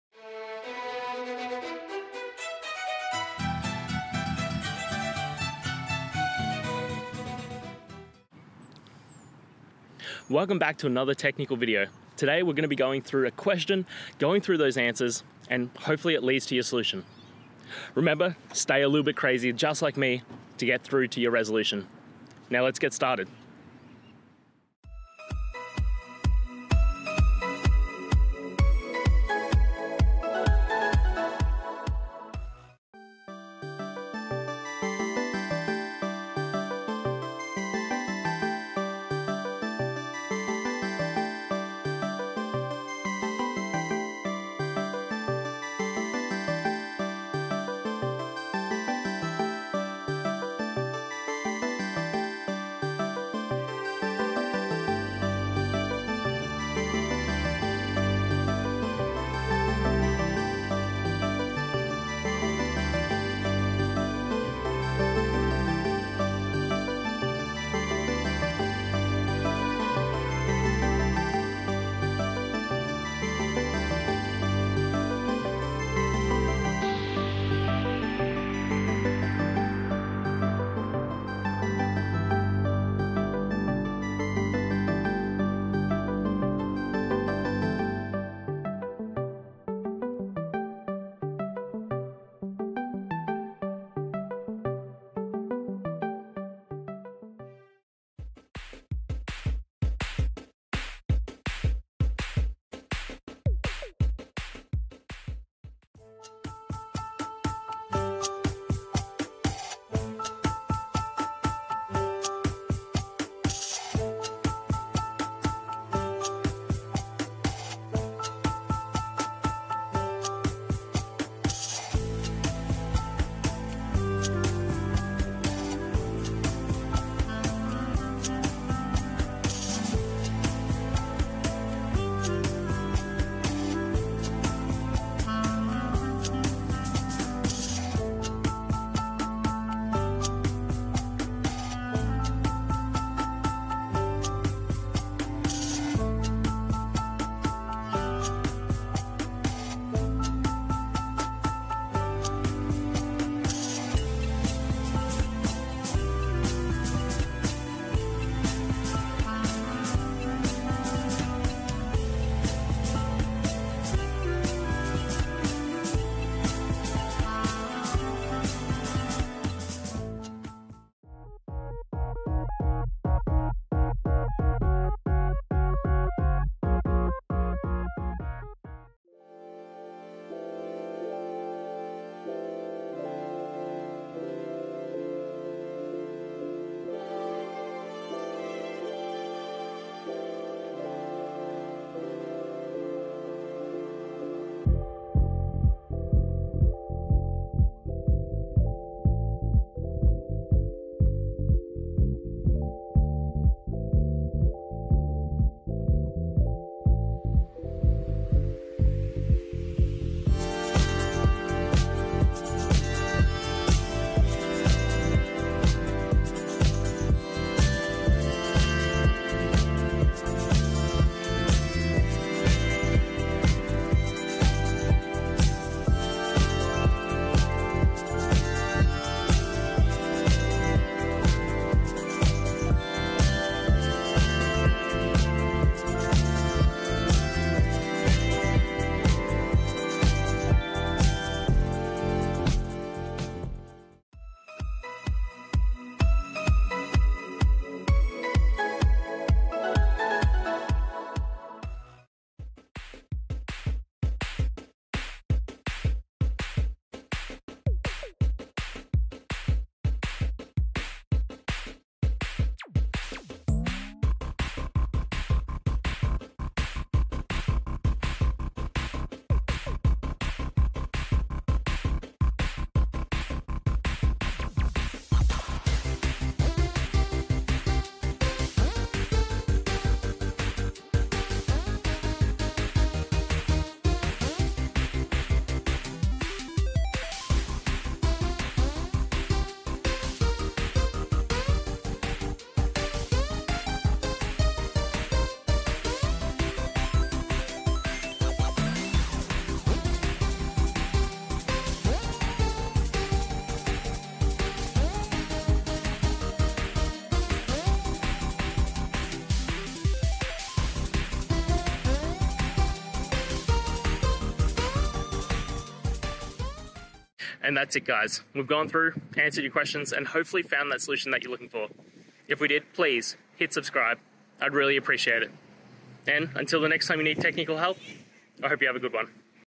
Binary Mask Shift Operation Without Cycle: Solutions and Techniques Explained
In this video, we delve into the fascinating world of binary mask shift operations, exploring innovative solutions and techniques that eliminate the need for cycles. Whether you're a seasoned programmer or just starting out, understanding these operations is crucial for optimizing performance in various applications. Join us as we break down complex concepts into easy-to-understand explanations, providing you with the tools to enhance your coding skills and tackle challenges with confidence.

Today's Topic: Binary Mask Shift Operation Without Cycle: Solutions and Techniques Explained

Related to